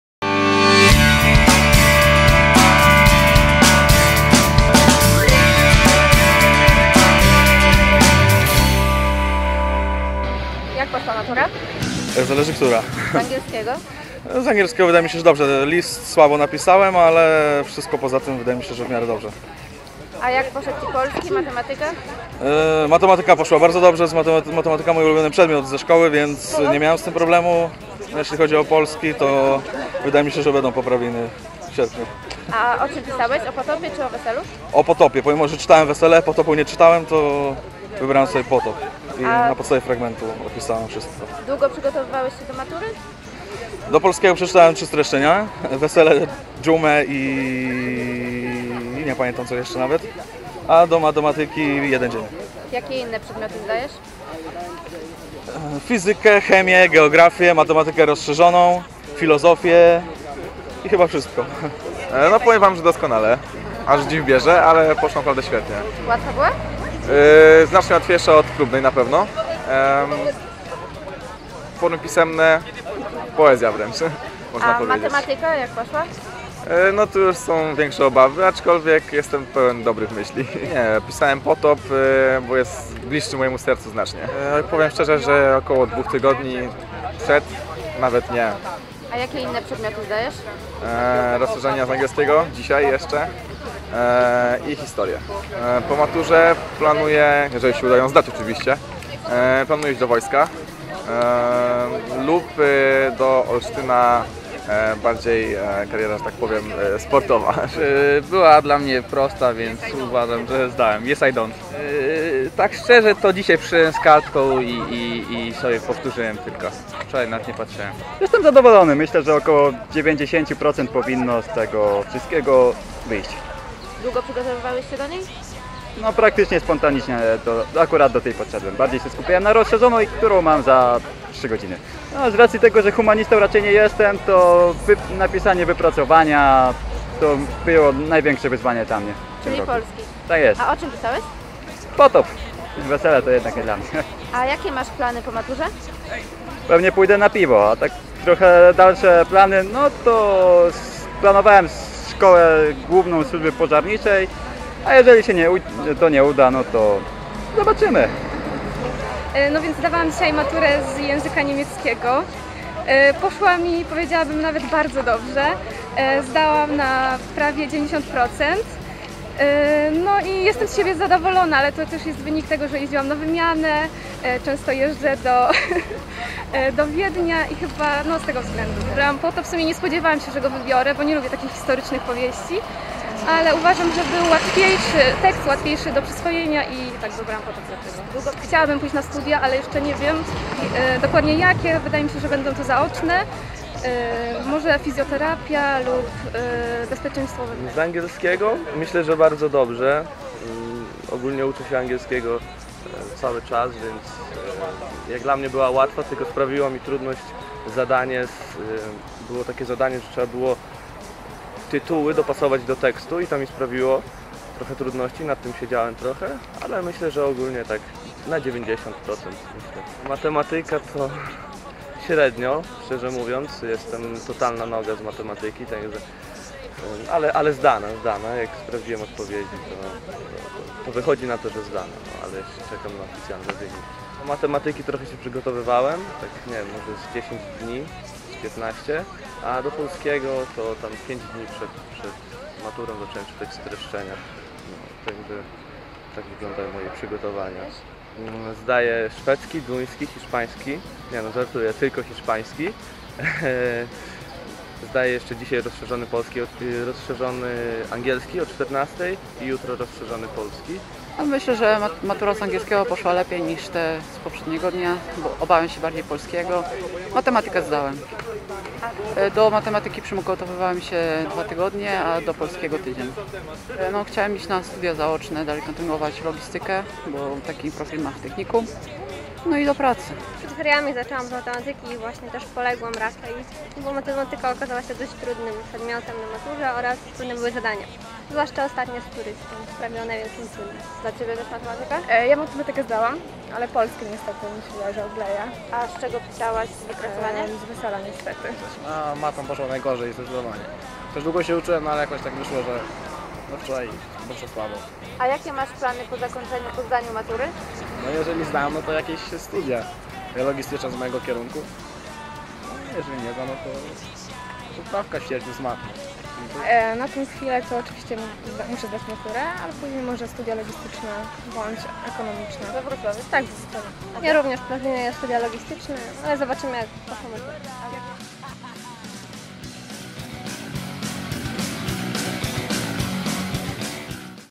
[SONDA] Oława - Który język obcy wybrać? Sonda oławskich maturzystów
Dziś z kamerą odwiedziliśmy Liceum Ogólnokształcące Nr 2 im. Zjednoczonej Europy w Oławie, gdzie od godziny 9:00 maturzyści pisali obowiązkowy przedmiot jakim jest język angielski na poziomie podstawowym.